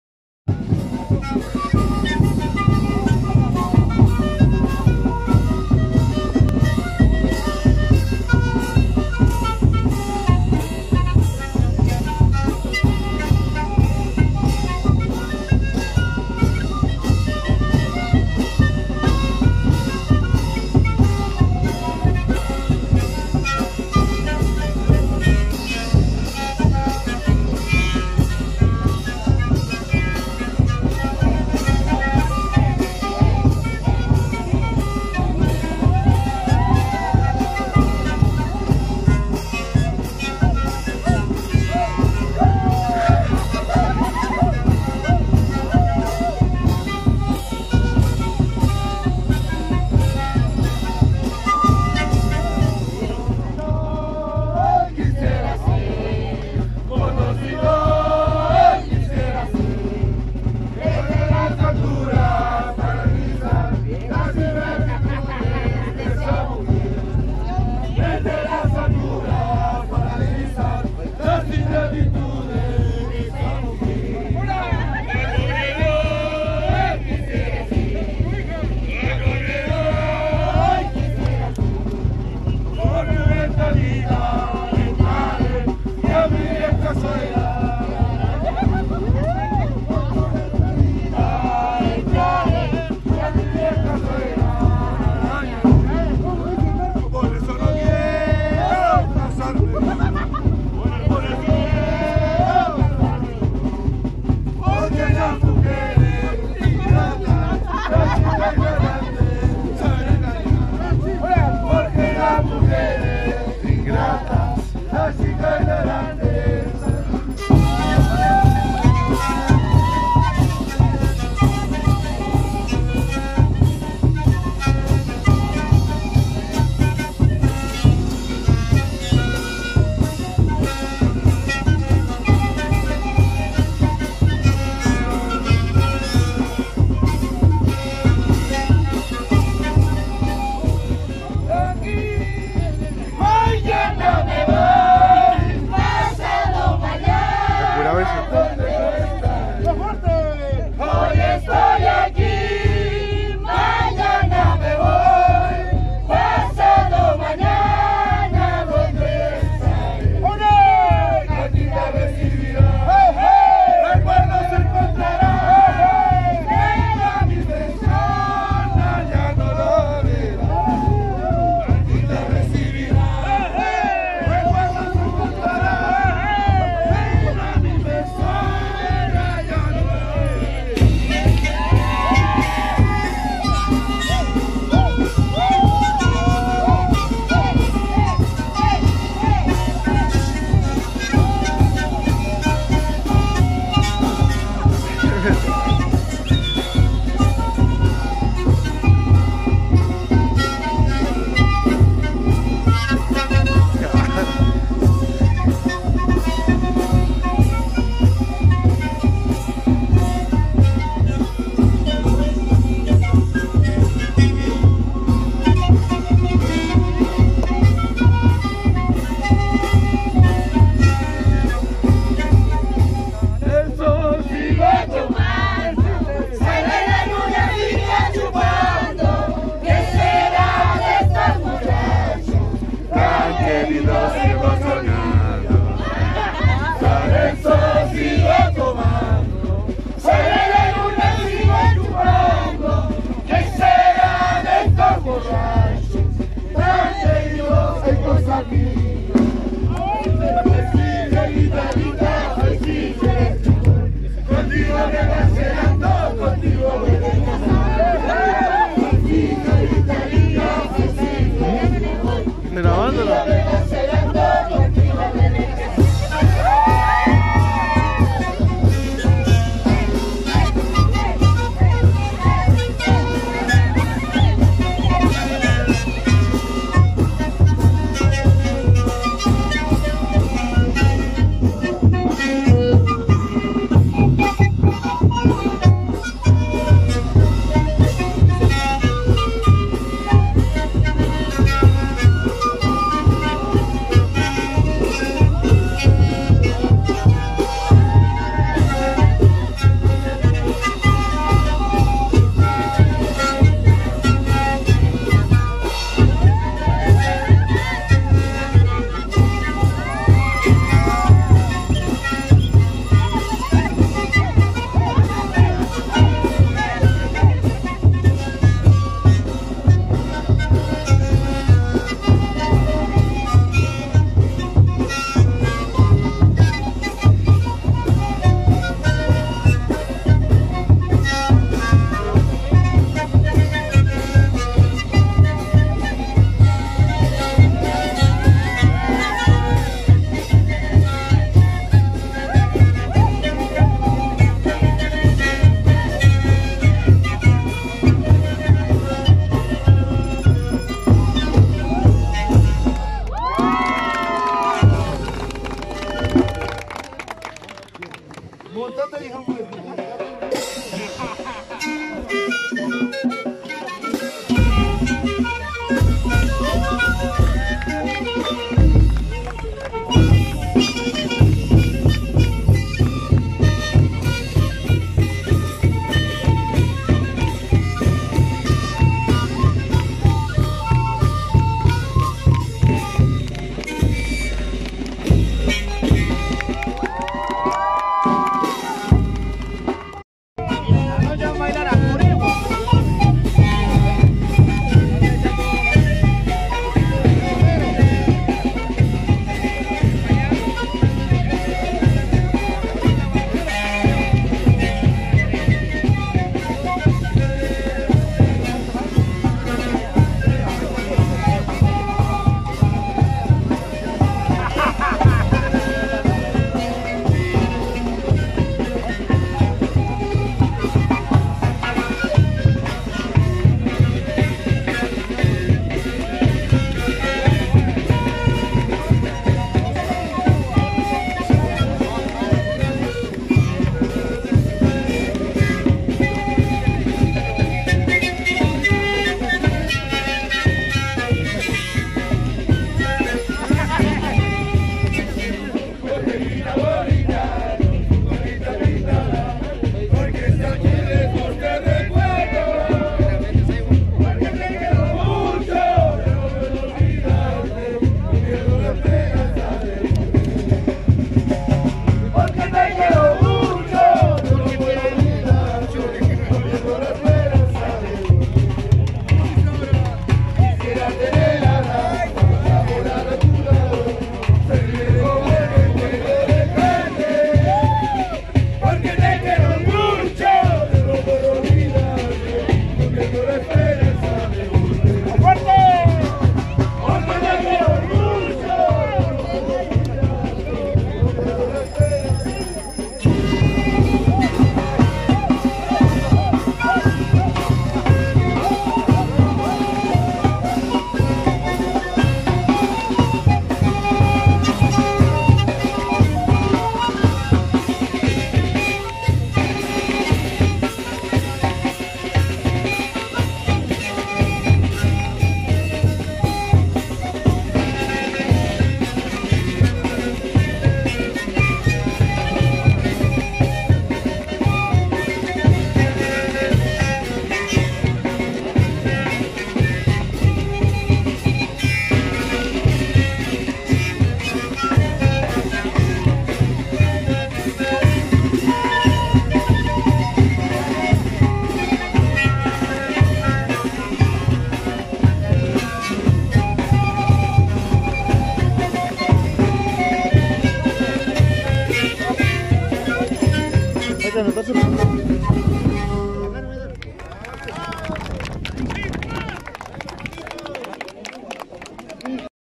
Todo los Santos pueblo cosapilla
Todo los Santos cementerio del pueblo de cosapilla año 2019